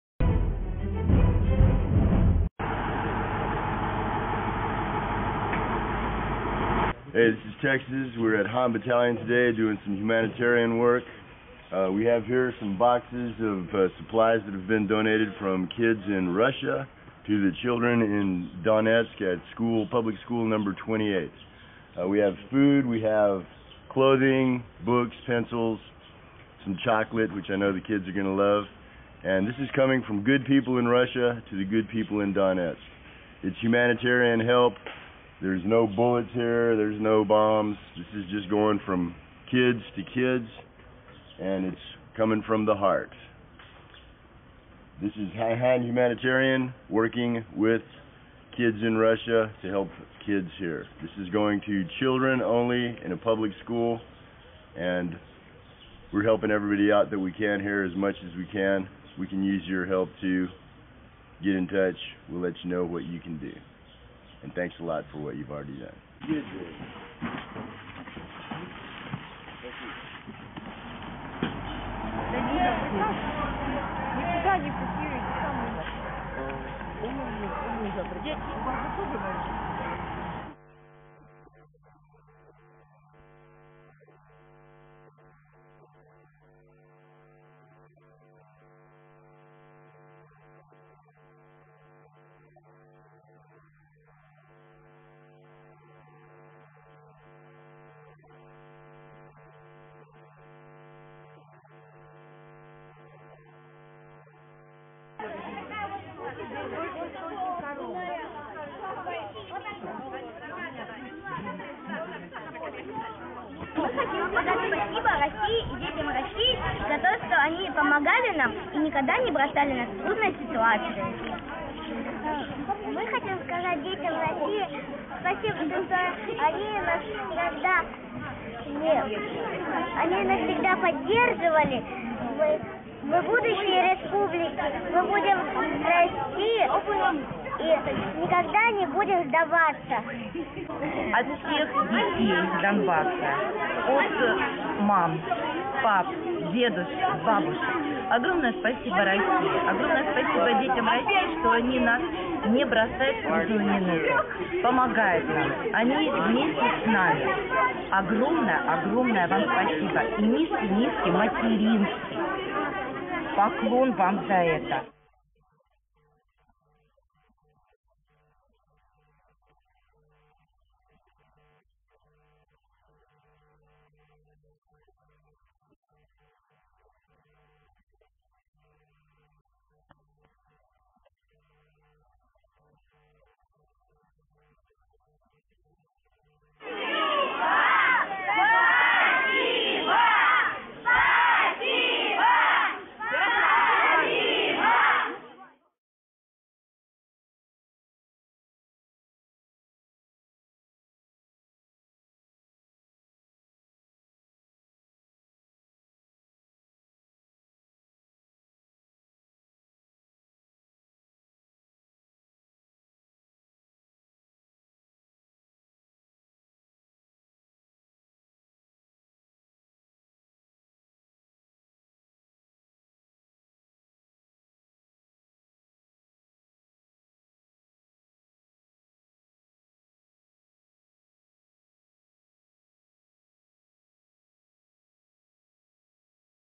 "Texas" et le bataillon "Khan" : aide humanitaire aux enfants de Donetsk
Donetsk, le 17 juin 2015. Le citoyen américain "Texas" qui se bat du côté des forces républicaines pour la liberté du Donbass achemine l'aide humanitaire envoyée par les enfants de la Fédération de Russie et appelle à l'aide.